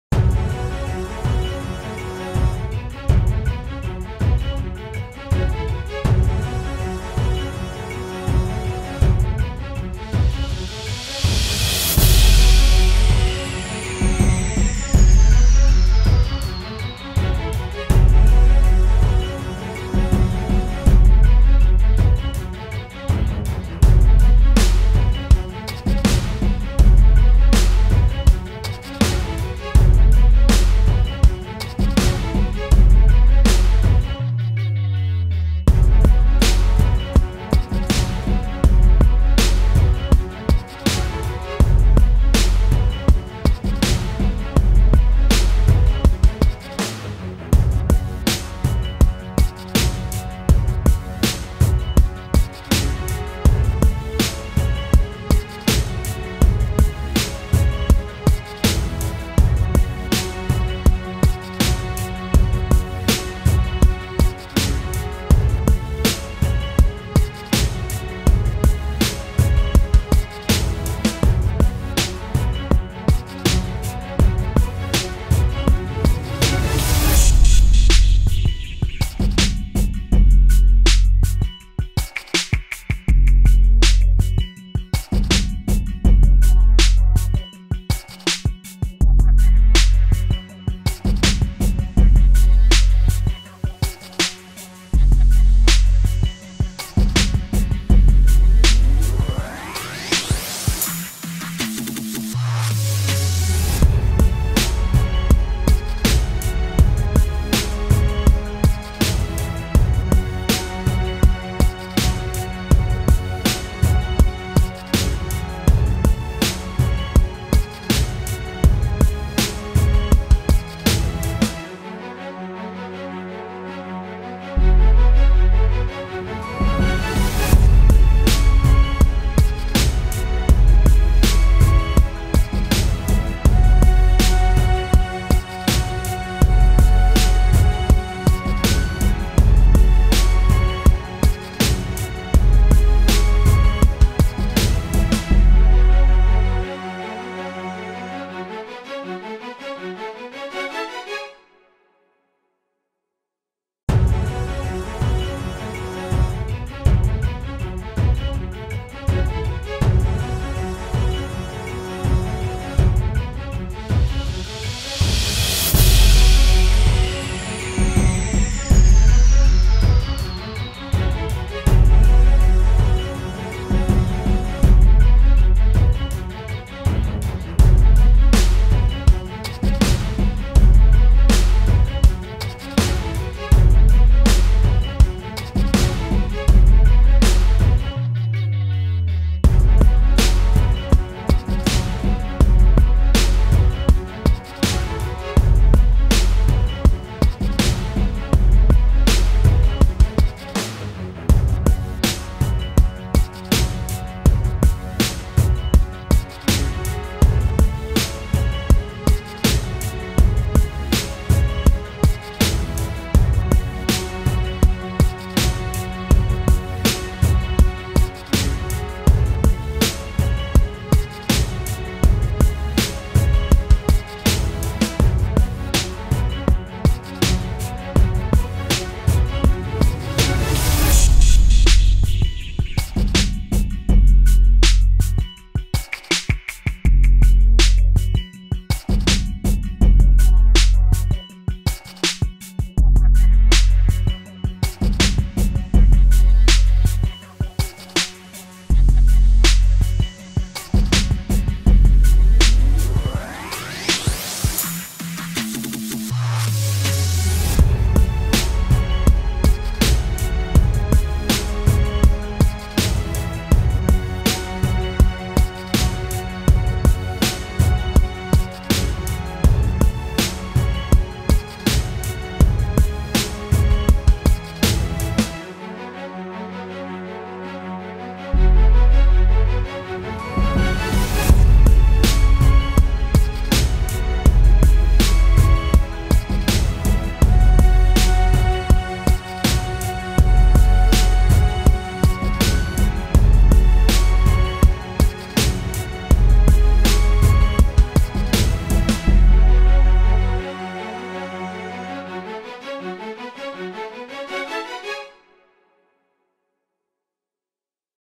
Desenhando Yennefer com as duas mãos - The Witcher Netflix
Desenhando a Yennefer de The Witcher da Netflix, utilizando as duas mãos!